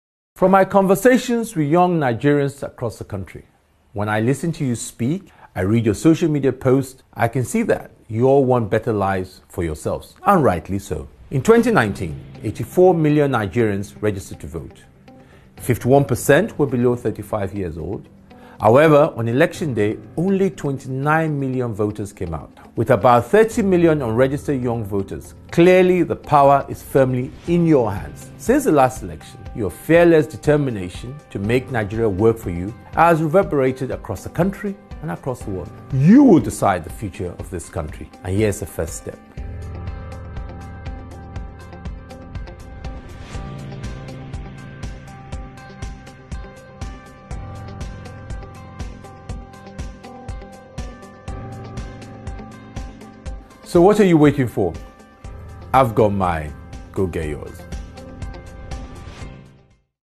Dr. Bukola Saraki urges youths to get registered as INEC commenced registration.
bukolasaraki The power to change Nigeria is in your hands. In 2019, only 29 million Nigerians came out to vote. Yet, over 30 million young people have not even registered to vote.

INEC’s voter registration process begins today.

I’ve got mine. GoGetYours!